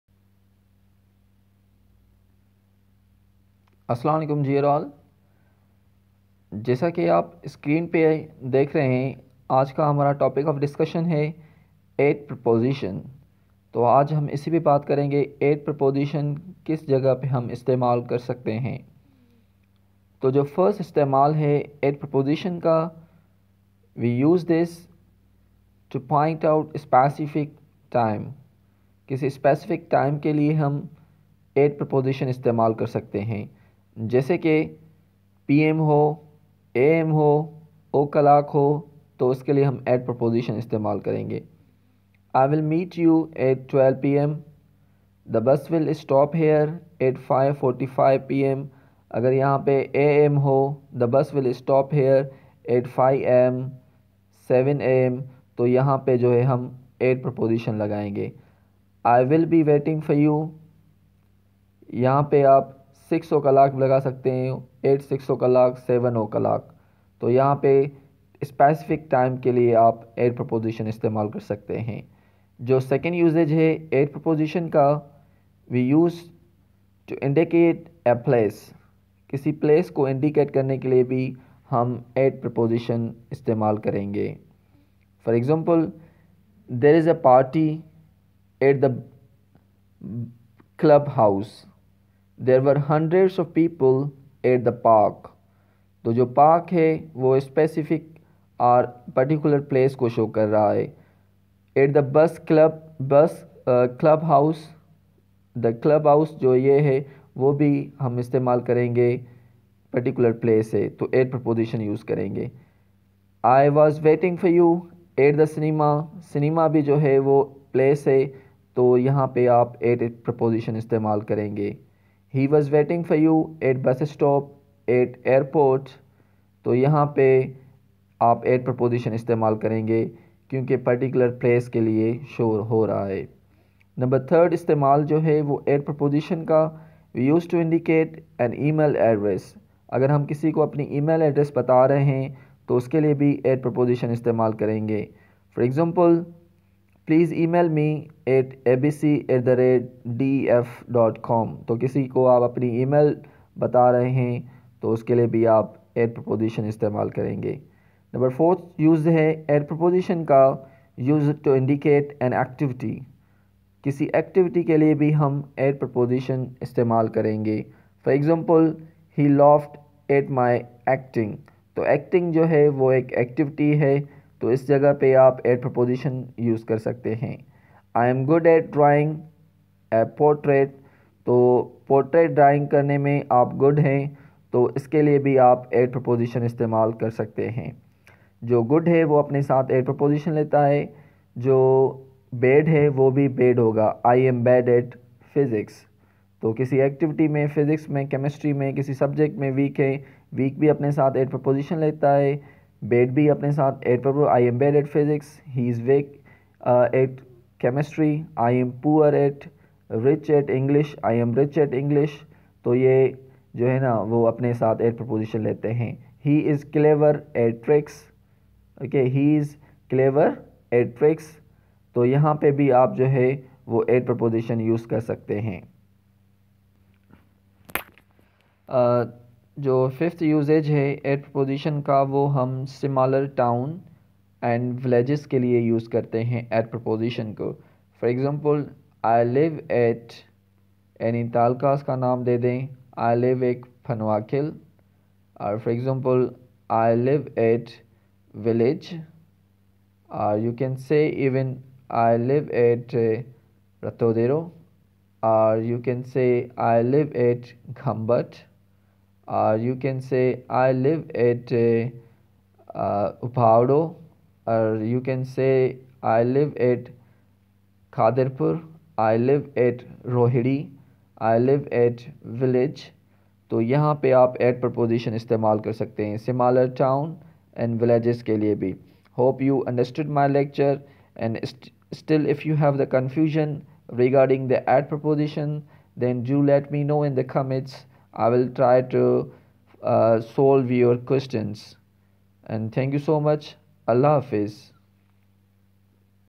Use of 'AT' Preposition in URDU and HINDI
We will learn where and how to use 'at' preposition conceptually.